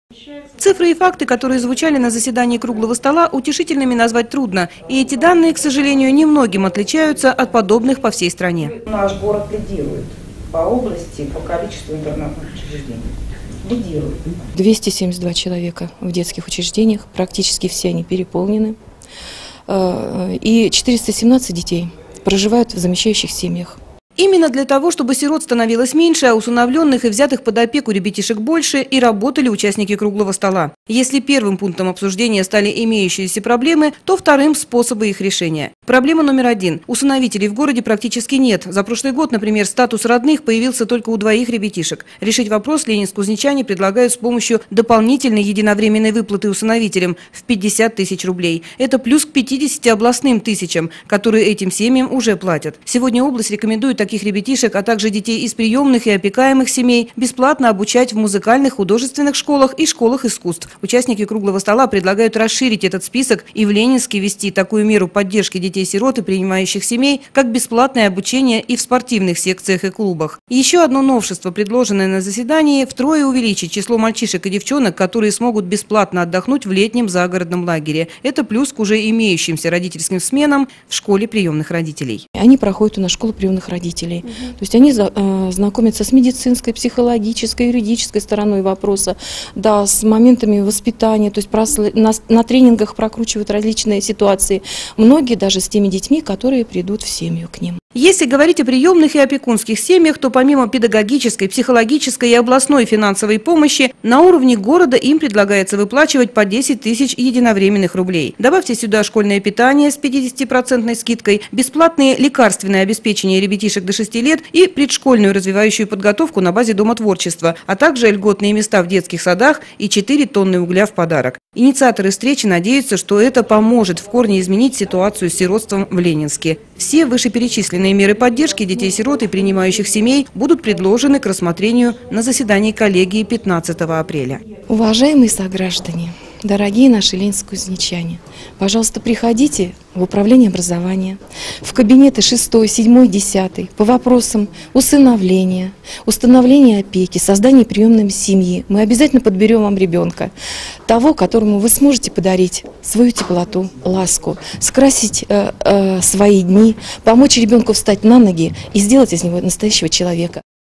Меры поддержки детей-сирот и принимающих семей
Тема социального сиротства сегодня -- на повестке дня всего российского общества. О том, какие меры поддержки ребятишек из детских домов и интернатов сегодня уже работают, и какой комплекс муниципальных мер для детей и принимающих семей предлагают участники прошедшего круглого стола по вопросам социального сиротства, - в сюжете Ангелики Вольф.